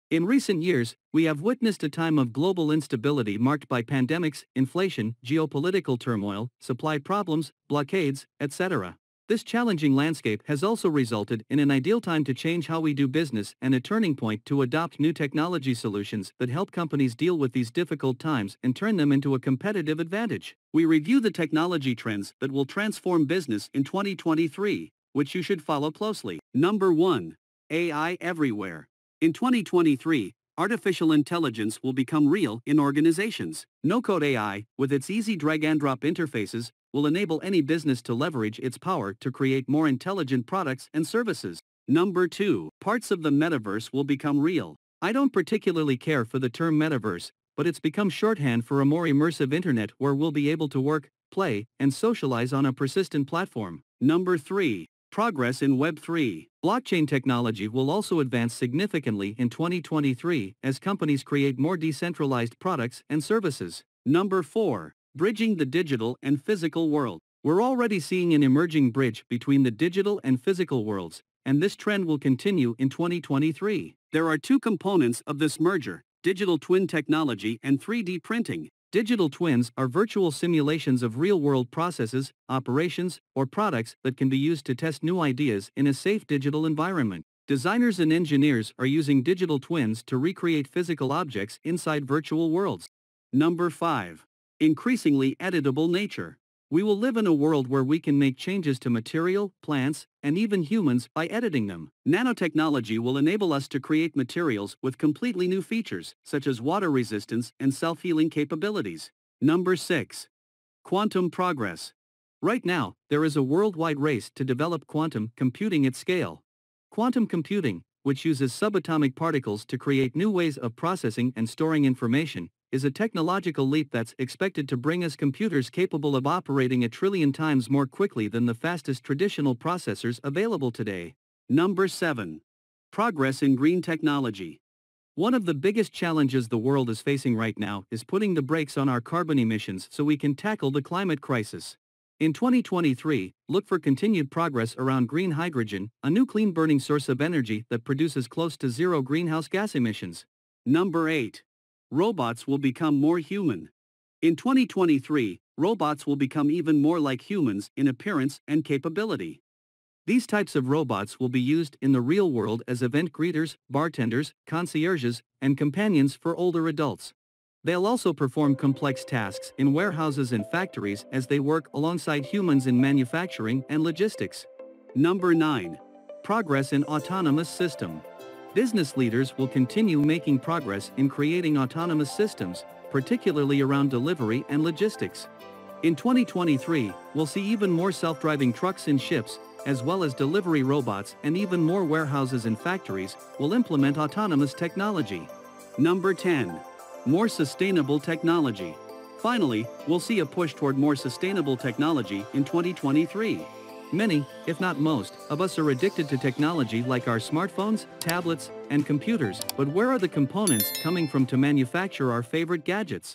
Top 10 Breakthrough Technologies of 2023: What's New and Exciting
Keep up with the times with the top 10 revolutionary technologies of 2023. We'll examine the newest and greatest technological developments in this video, from cutting edge AI to ground breaking new gadgets. We'll offer you a detailed look at what's new and interesting in the IT world and give you the knowledge you need to remain on top of things. This video is a must watch whether you're a tech fanatic or you're just interested in what the future holds. The top 10 technologies of 2023 will inspire you, so get ready.